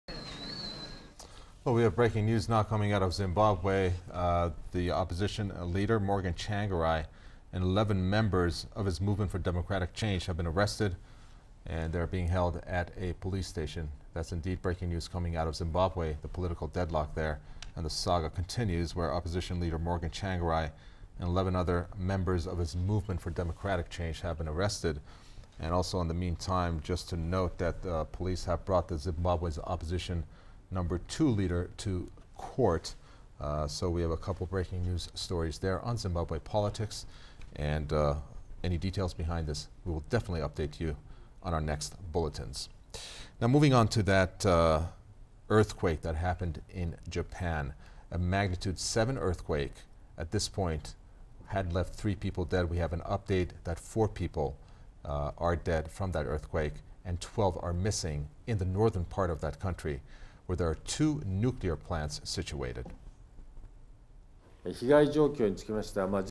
Kaveh Taghvaie, Breaking News, Zimbabwe
Press TV's Kaveh Taghvaie presents breaking news on Zimbabwe political turmoil